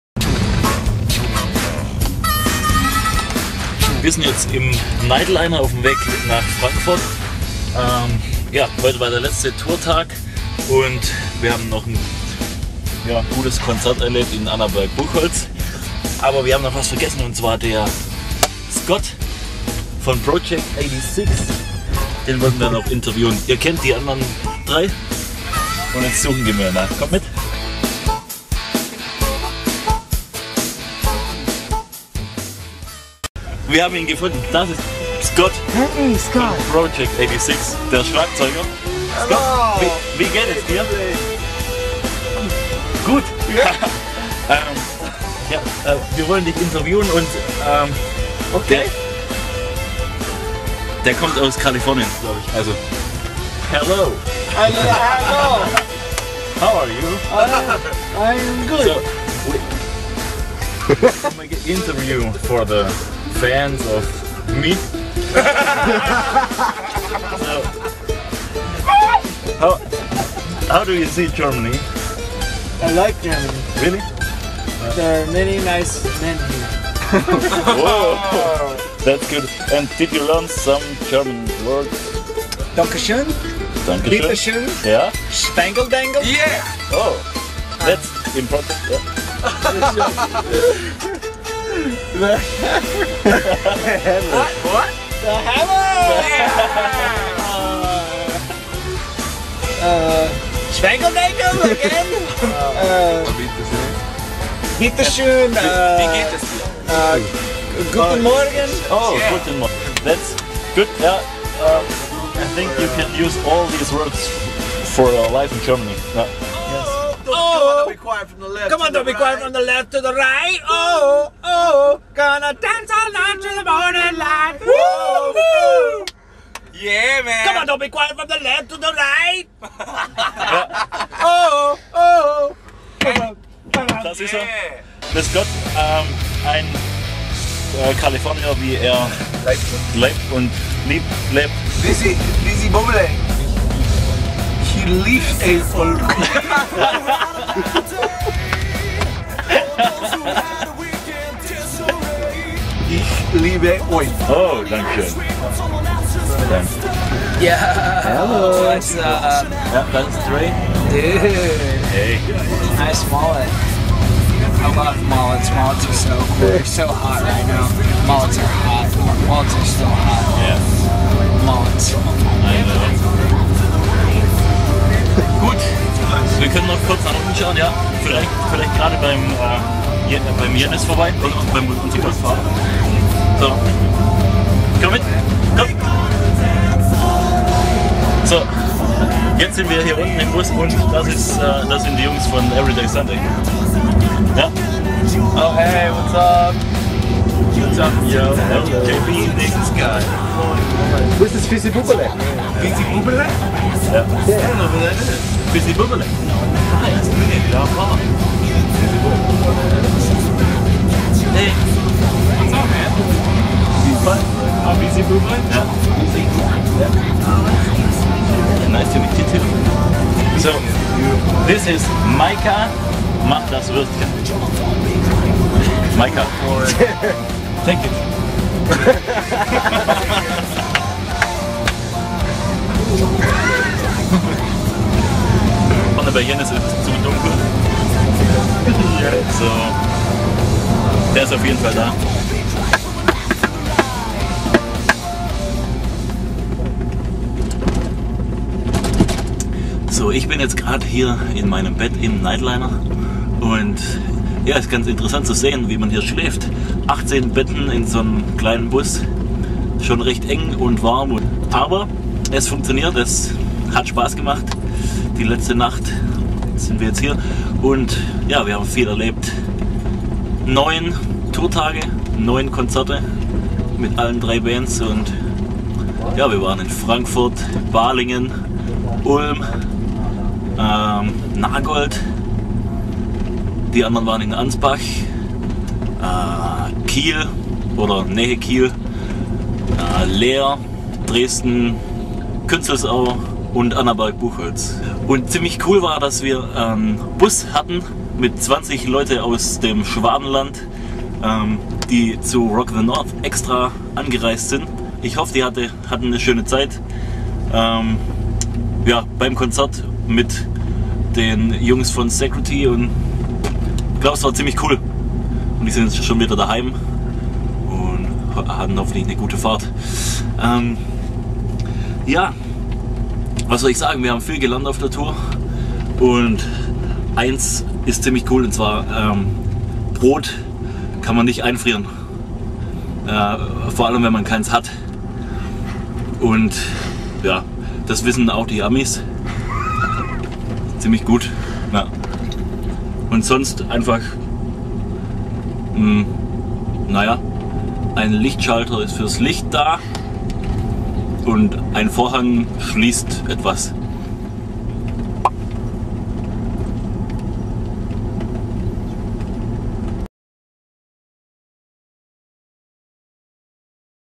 RYSO Tour Fizzy Teil 2
Fizzy Bubbele nimmt euch mit auf die Rock Your Soxx off Tour!!! Sacrety, Project 86 und Everyday Sunday!